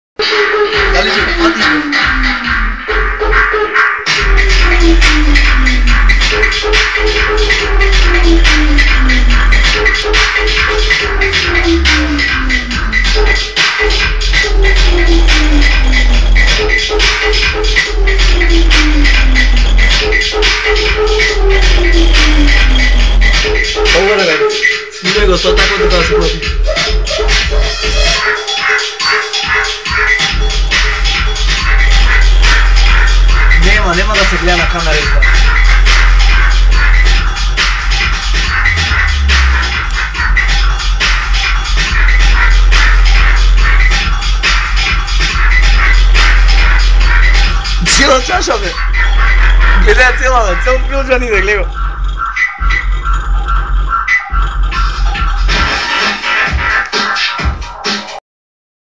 trese bas Hrom
casa od kafe so lazice se trese od surround